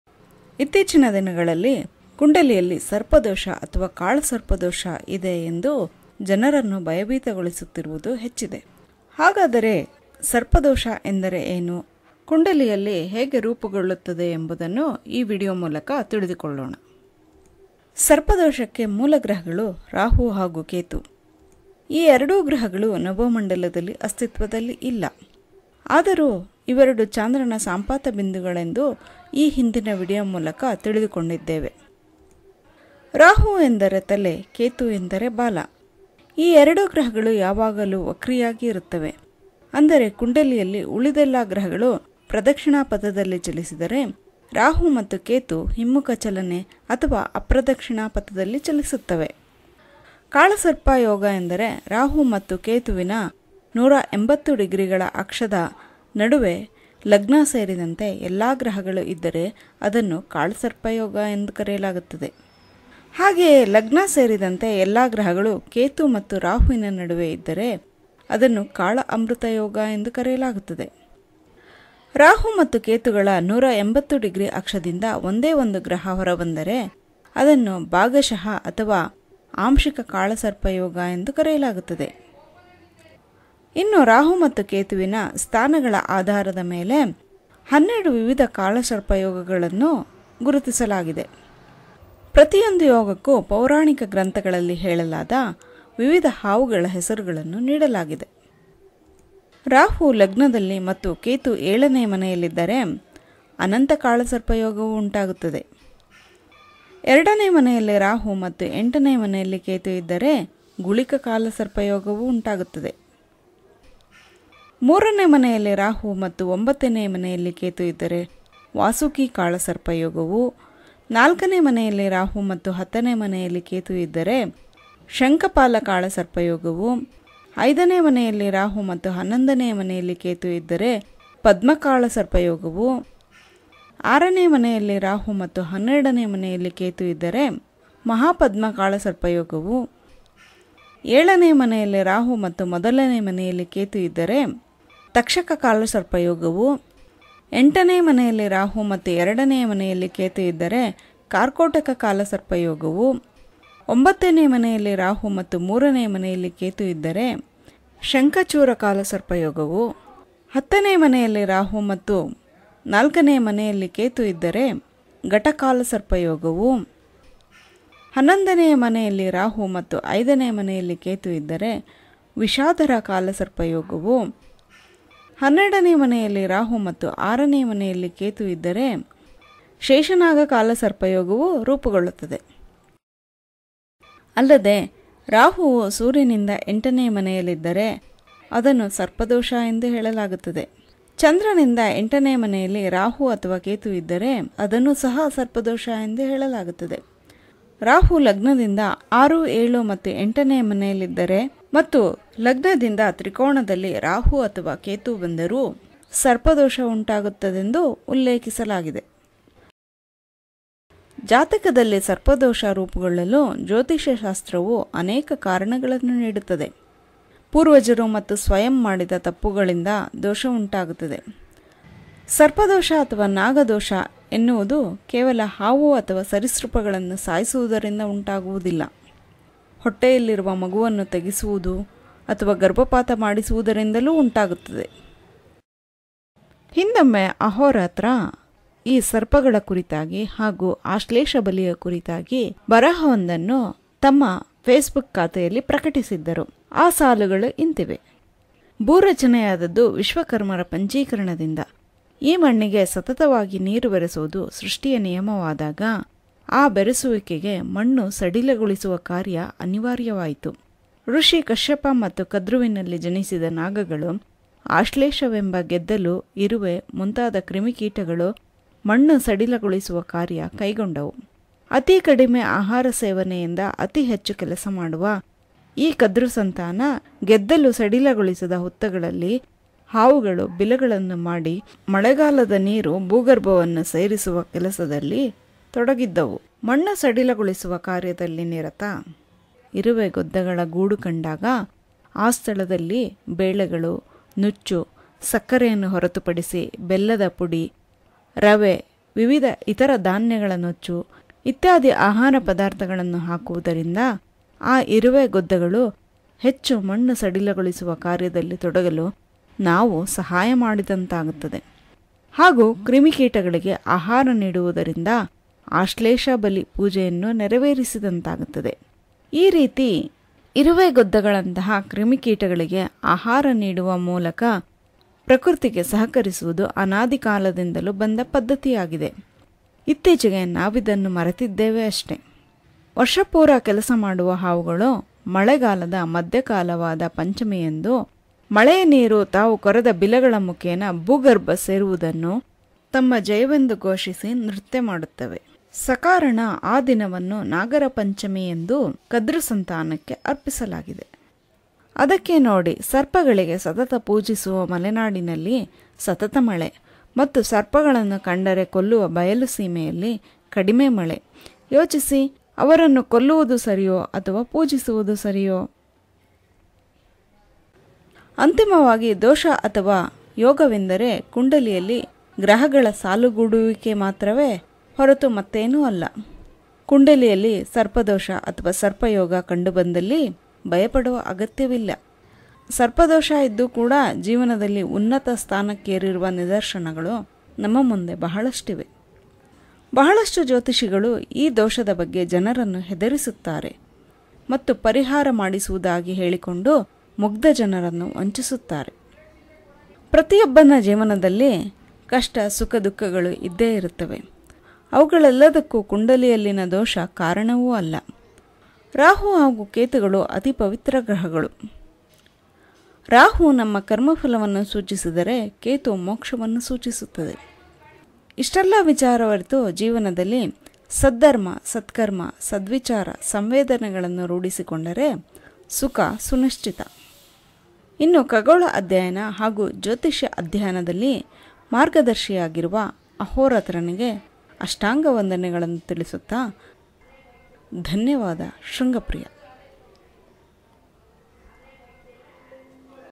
ಕುಂಡಲಿಯಲ್ಲಿ ಸರ್ಪದೋಷ ಪ್ರಕಟವಾಗಿದ್ದರೂ ಸಹ ಭಯಪಡುವ ಅಗತ್ಯವಿಲ್ಲ..! - ಶೃಂಗಪ್ರಿಯ
ಪ್ರತಿಯೊಬ್ಬ ವ್ಯಕ್ತಿಯ ಕುಂಡಲಿಯಲ್ಲಿ ಒಂದಲ್ಲ ಒಂದು ಯೋಗ ದೋಷಗಳು ಉಂಟಾಗುತ್ತವೆ, ಅಂತೆಯೇ ಸರ್ಪದೋಷ ಸಹ ಸಾಮಾನ್ಯವಾಗಿ ಕಂಡುಬರುತ್ತದೆ. ತಮ್ಮ ಕುಂಡಲಿಯಲ್ಲಿ ಸರ್ಪದೋಷ ಪ್ರಕಟವಾಗಿದ್ದರೂ ಸಹ ಭಯಪಡುವ ಅಗತ್ಯವಿಲ್ಲ.
- ಶೃಂಗಪ್ರಿಯ